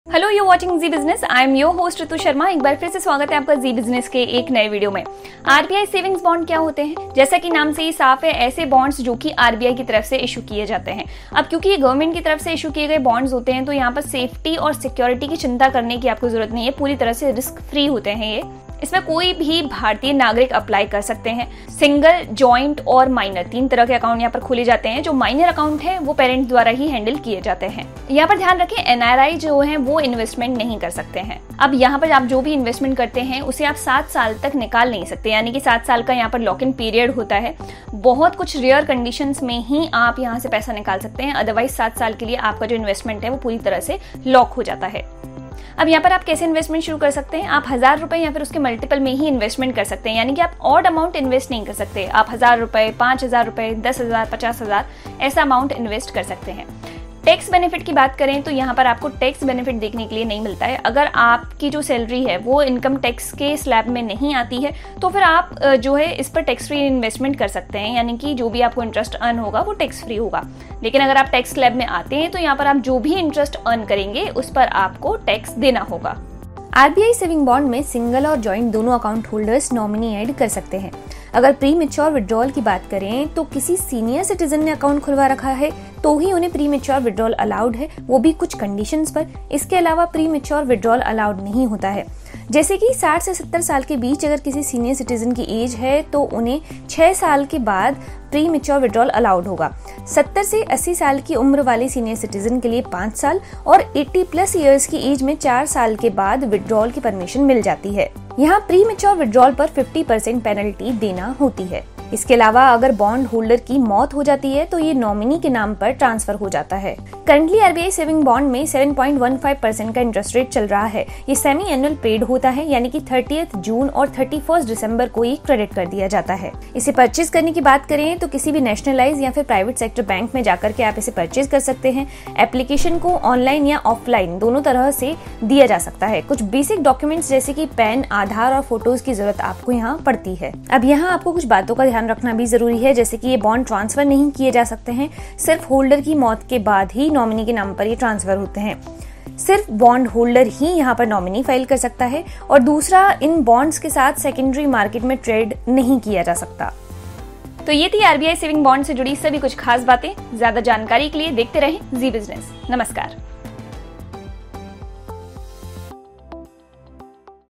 RBI सेविंग्स बॉन्ड क्या हैं, कैसे कर सकते हैं इनमें निवेश- जानें डिटेल्स | Zee Business
जैसा कि नाम से ही साफ होता है ऐसे बॉन्ड जो कि रिजर्व बैंक ऑफ इंडिया की तरफ से इश्यू कराए जाते हैं. क्योंकि ये सरकार की तरफ से पेश किए जाते हैं इसलिए पूरी तरह से सिक्योर होते हैं.
As the name suggests, such bonds are issued by the Reserve Bank of India. Because they are offered by the government, they are completely secure.